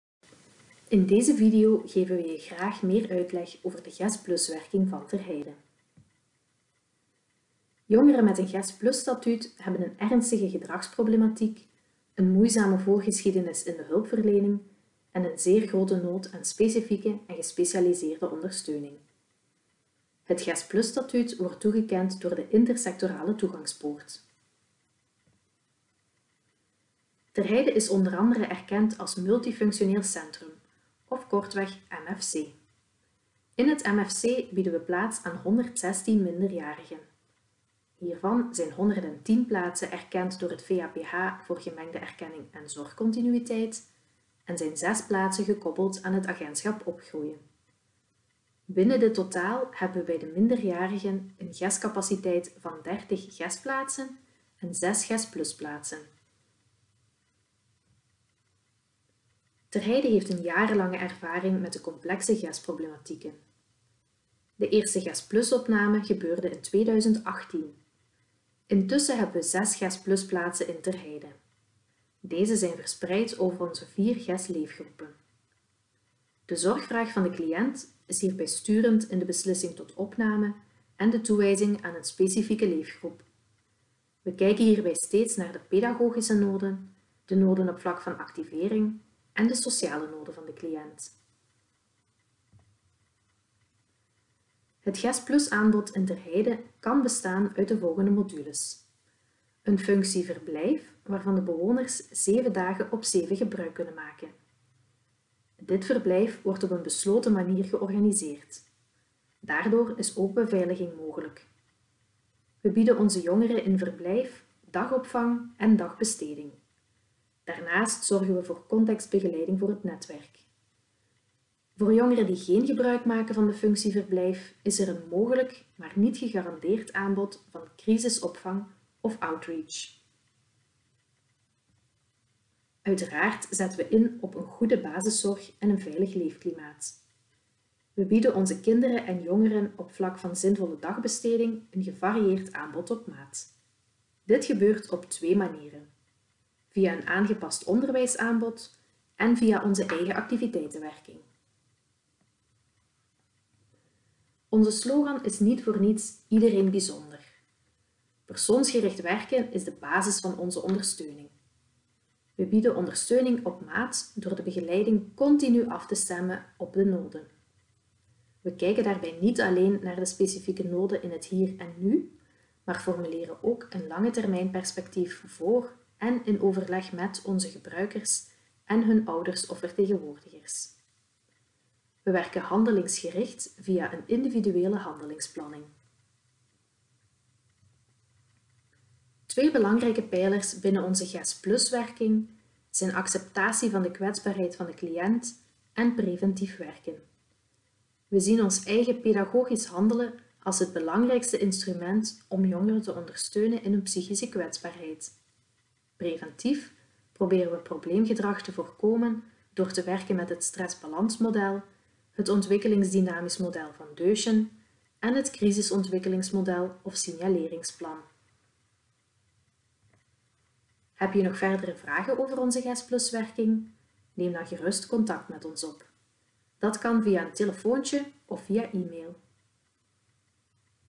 GES+-werking van Ter Heide
Op 1 januari 2018 werden we als GES+-voorziening erkend en in maart verwelkomden we de eerste nieuwe bewoner. Benieuwd wat de GES+-werking in Ter Heide precies inhoudt? We vatten samen in deze korte video.